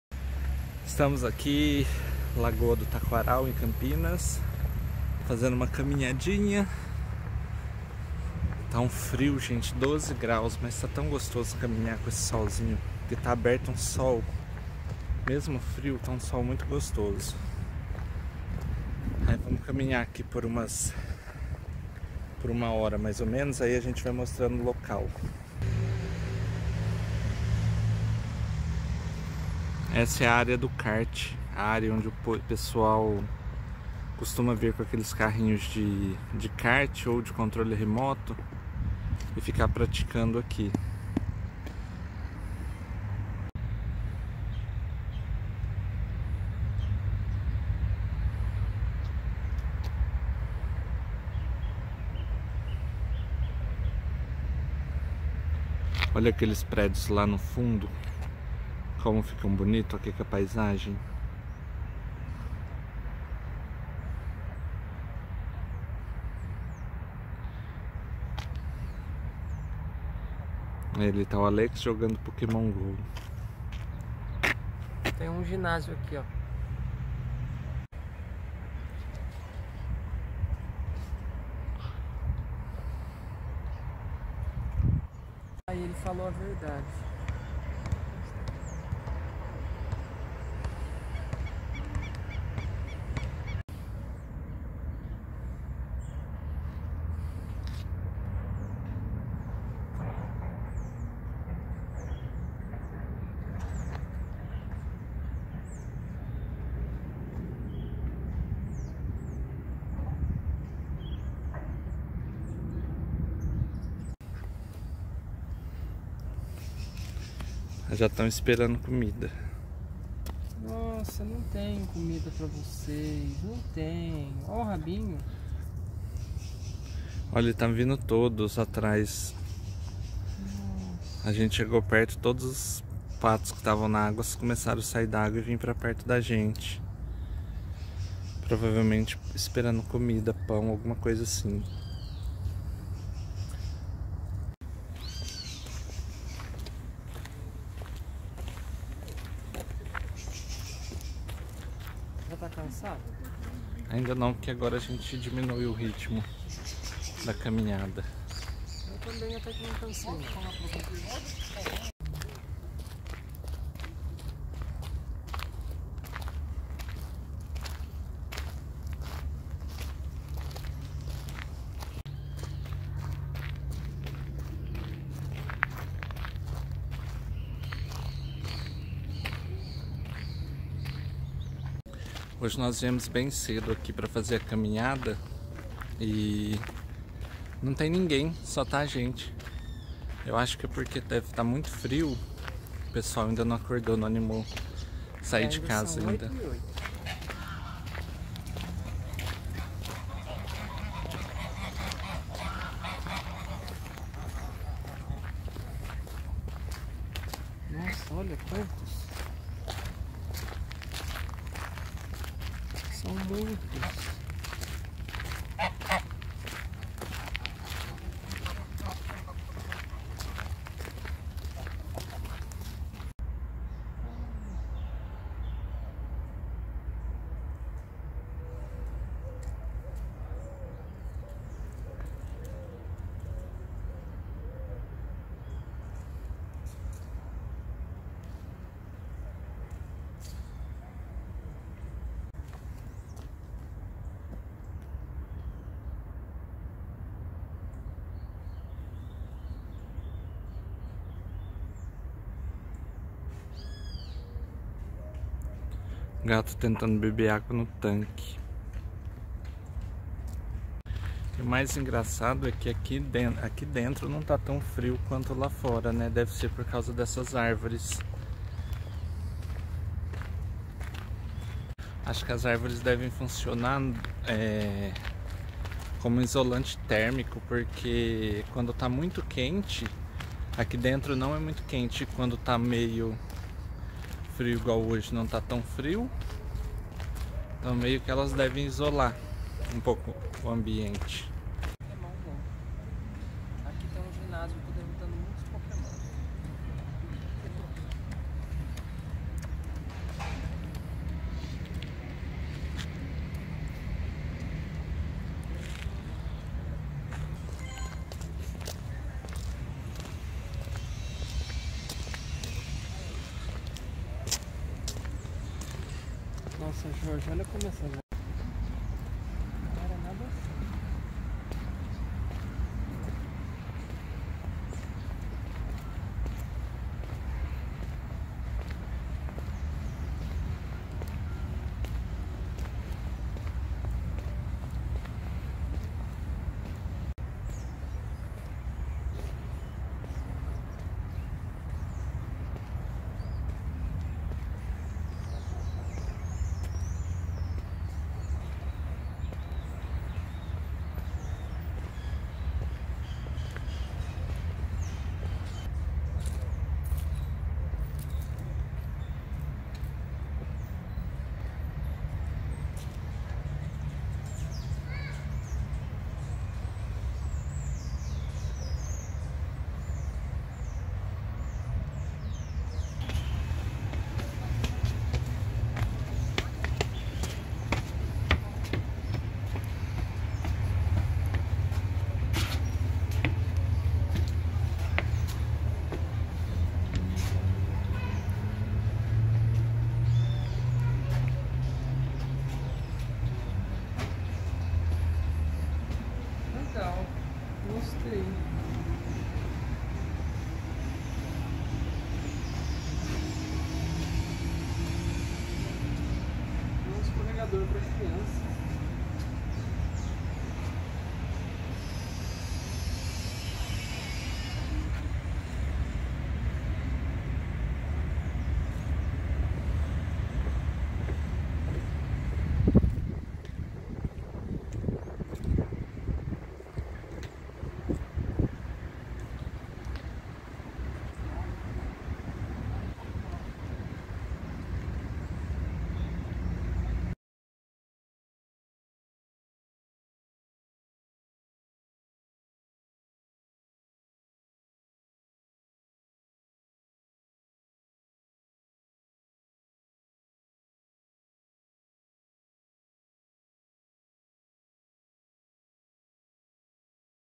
CAMPINAS-SP | Lagoa do Taquaral
O Parque Portugal (conhecido popularmente como Lagoa do Taquaral ou Parque Taquaral) é um parque no bairro do Taquaral, na Região Leste de Campinas, no estado de São Paulo, no Brasil. É o parque mais famoso da cidade. Junto com o Lago do Café e a Praça Arautos da Paz, forma a mais importante zona de lazer da cidade. O parque recebe diariamente milhares de pessoas, entre moradores da cidade e turistas, principalmente aos finais de semana. No verão, o parque chega a receber 50 000 visitantes em uma semana.

Dentre as atrações do parque, destacam-se sua grande lagoa, uma réplica da Caravela Anunciação, uma linha de bonde elétrico e uma concha acústica, além de pista de saibro para caminhada, ciclovia, cartódromo, quadras de futebol, basquete, vôlei e hóquei e uma ampla área verde. Aos domingos, entre 6 e 13 horas, a avenida ao redor do parque é fechada para veículos, ficando destinada ao ciclismo e lazer.